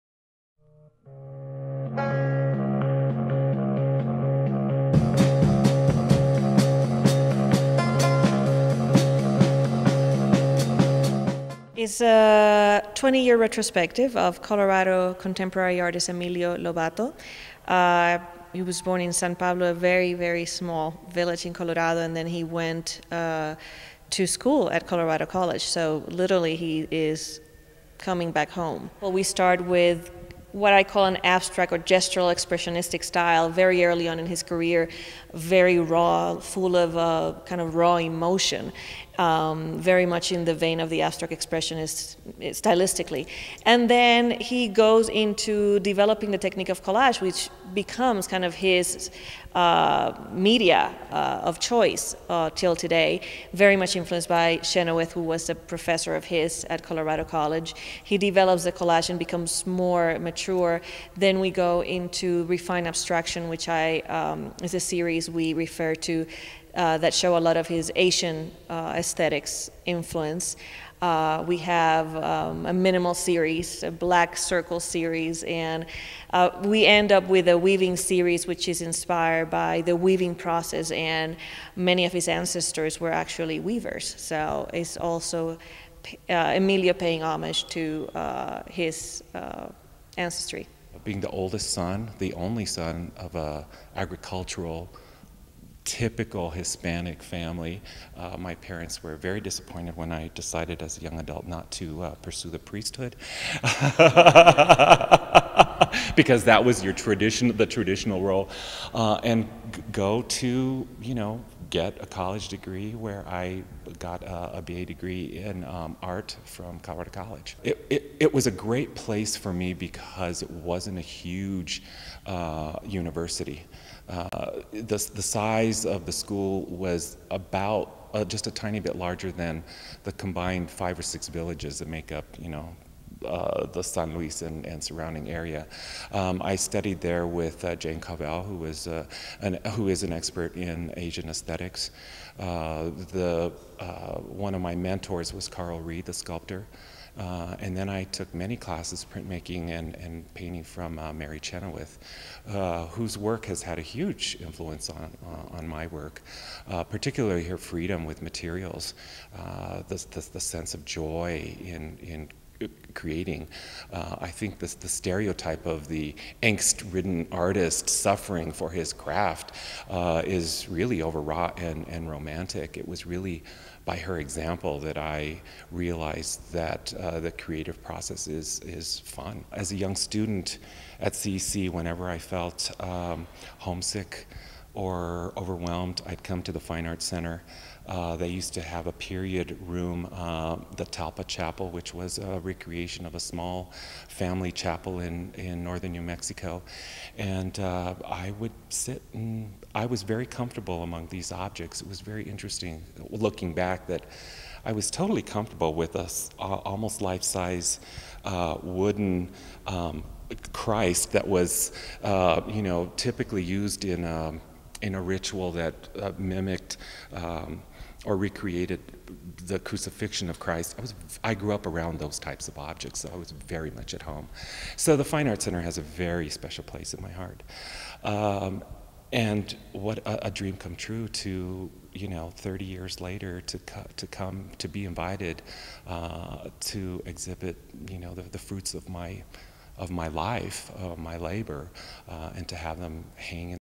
Mi Linda Soledad: Emilio Lobato's Retrospective at the FAC
Artist and Colorado College alumnus, Emilio Lobato, returns to his artistic roots in a career retrospective at the Colorado Springs Fine Arts Center.  In this video we speak with curator Tariana Navas-Nieves and the artist himself about the exhibit.